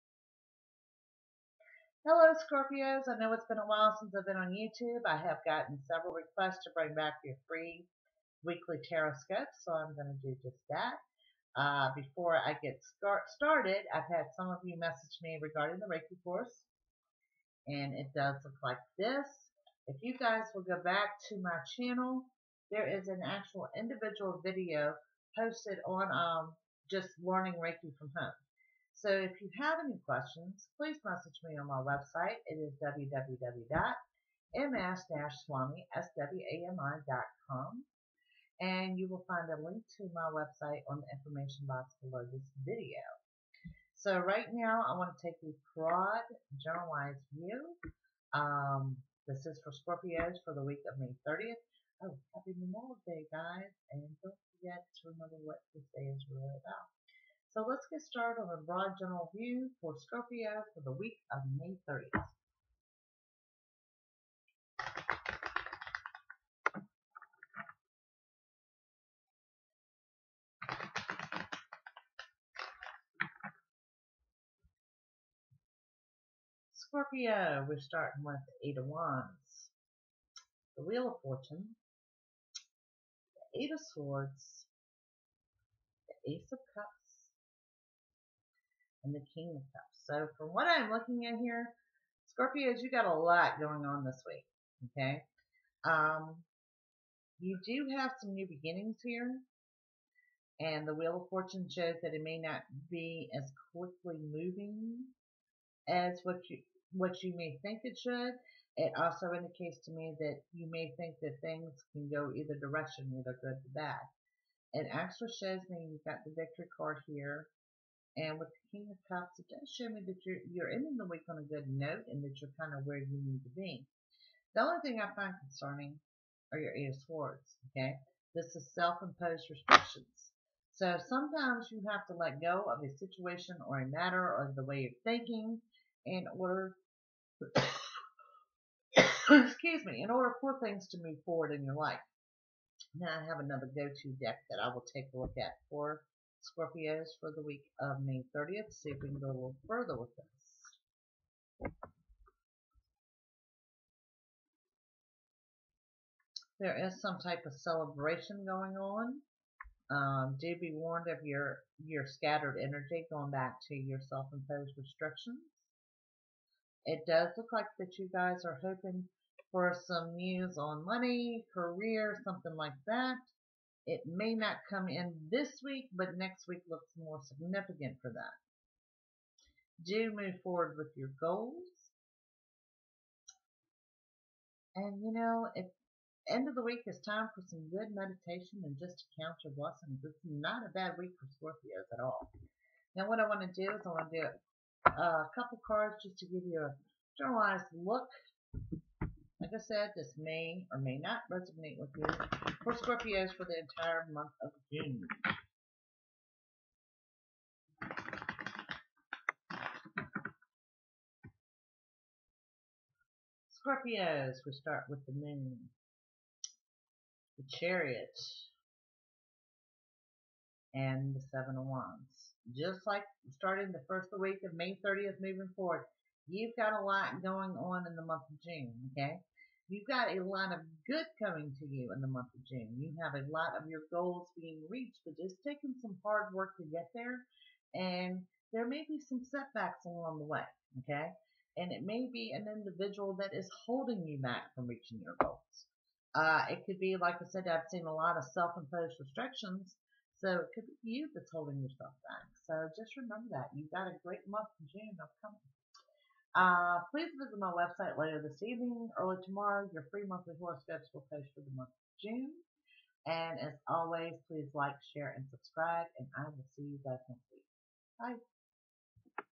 SCORPIO for the week of May 30 2016 with Jackie Tomlin
This is a broad generalized view for Scorpio for the week of May 30th 2016, and a glimpse of June 2016.

To contact Jackie directly, your free monthly horoscopes, Reiki and more please